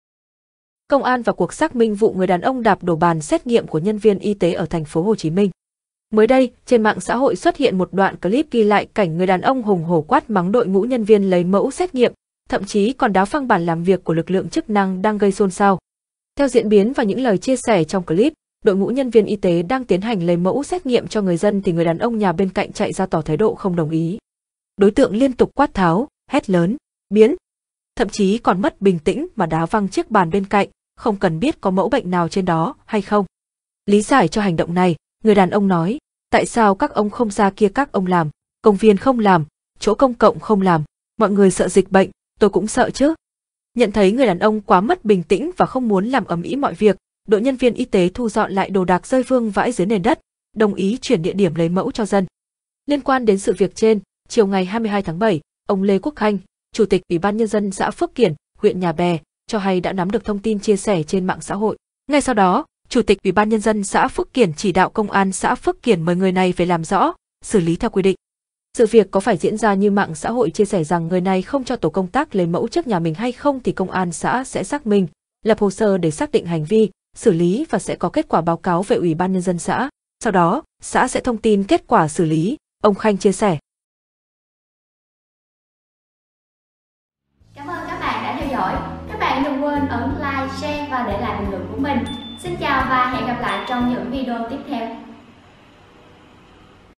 CĂNG ĐÉT: C.A vào cuộc vụ Ông Chủ Biệt Thự ĐẠP ĐỔ Bàn xét nghiệm Covid, đuổi nhân viên y tế
C.A vào cuộc vụ Ông Chủ Biệt Thự ĐẠP ĐỔ Bàn xét nghiệm Covid, đuổi nhân viên y tế